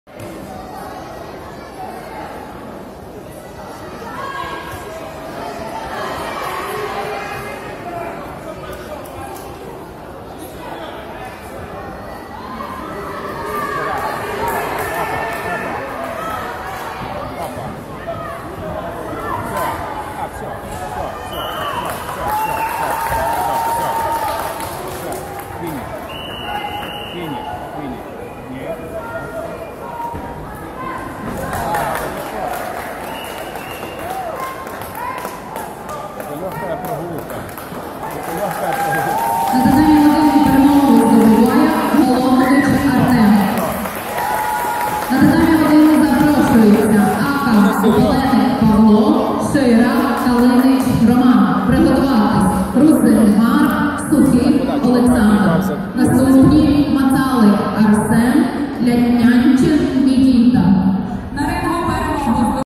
Гурнєв Олексій 1 бій.
Міжнародний турнір Шидокан Карате. Ужгород. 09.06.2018